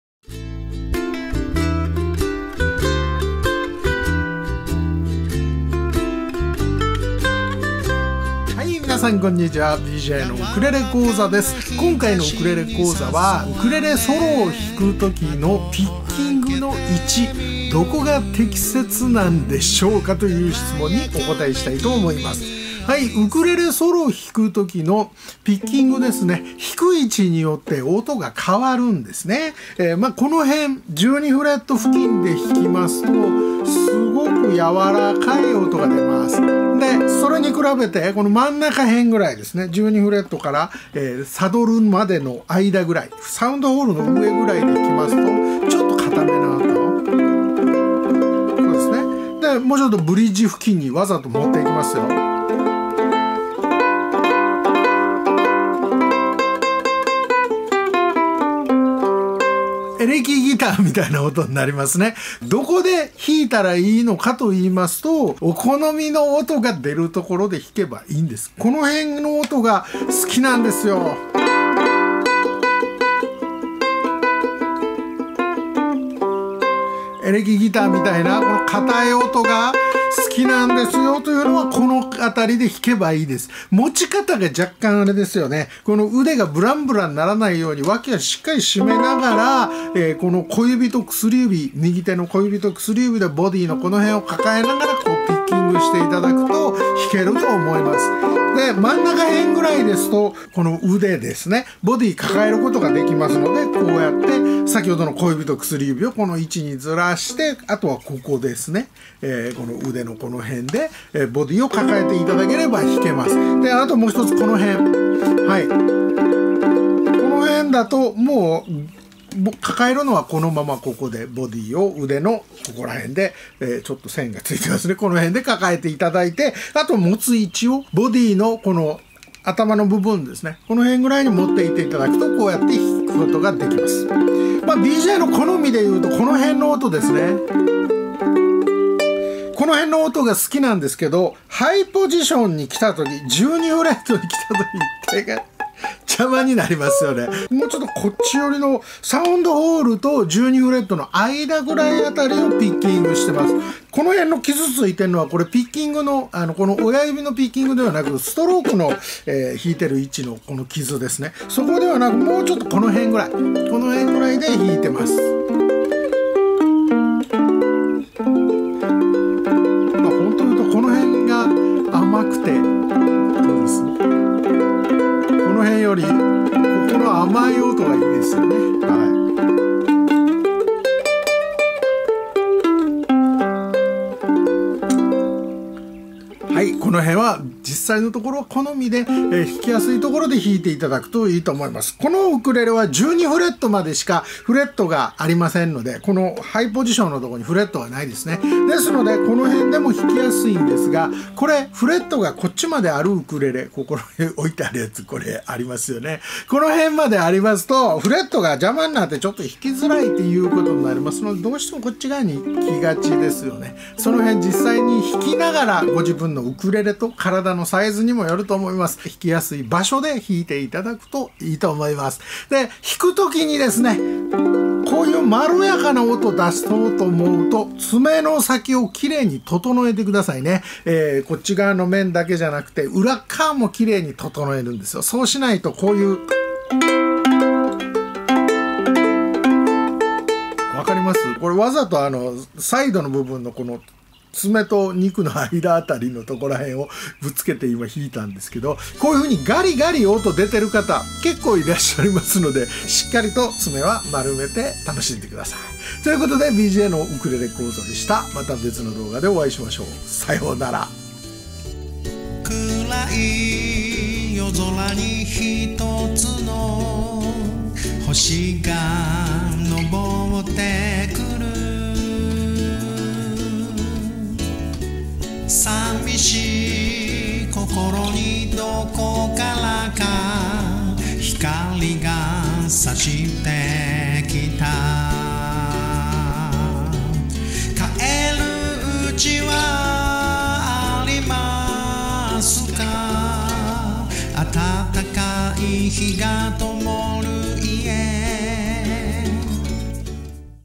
ウクレレソロのピッキングの適切な位置は？ ／ 【毎日更新】 BJのウクレレ講座 No.1842
ＢＪアレンジ譜面ダウンロード

D'Addario ダダリオ ブラックナイロン弦（サイズ選択ください）

SAVAREZ サバレス ウクレレ弦 Low-G用巻き弦（単品） 144RL